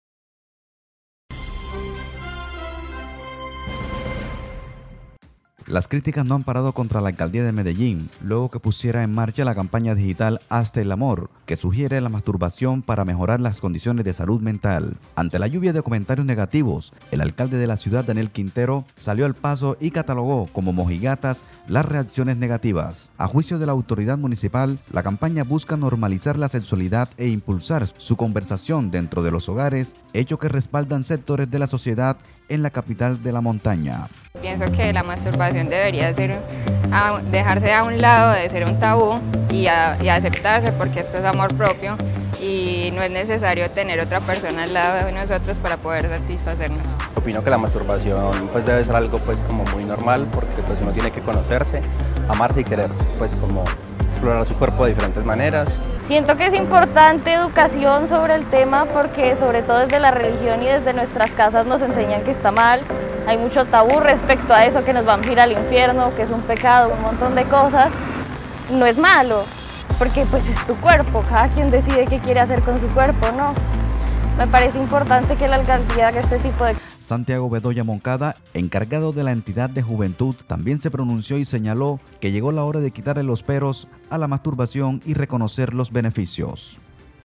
En Medellín lanzan la campaña de masturbación ‘Hazte el amor’ y las críticas no paran
Desde e este jueves, 17 de marzo, las críticas no han parado contra la Alcaldía de Medellín, la cual puso en marcha la campaña digital ‘Hazte el amor’ que sugiere la masturbación para mejorar las condiciones de salud mental.